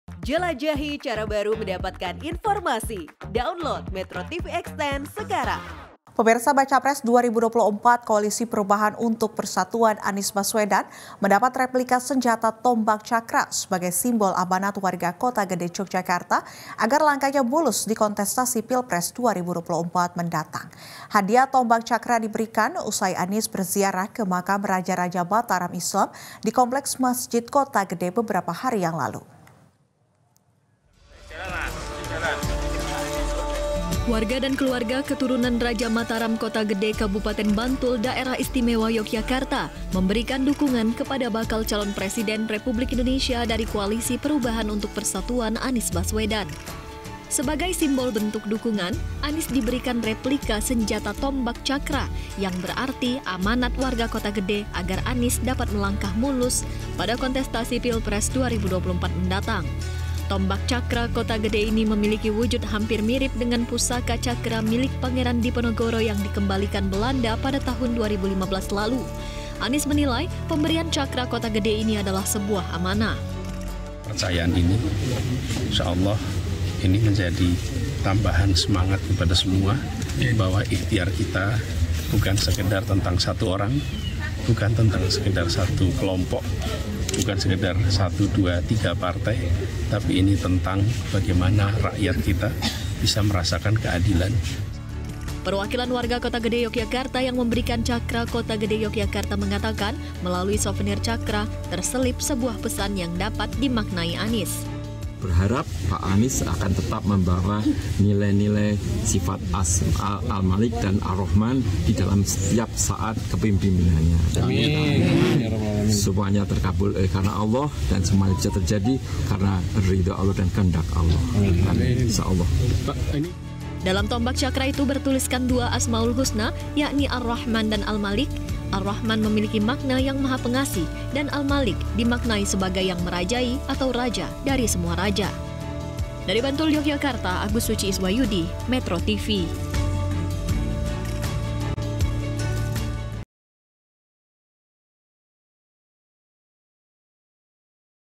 Replika Tombak Cakra Untuk Anies
METRO TV JATENG & DIY, Bacapres 2024 koalisi perubahan untuk persatuan Anies Baswedan mendapat replika senjata tombak cakra sebagai simbol amanat warga kota Yogyakarta agar langkahnya mulus kontestasi Pilpres 2024.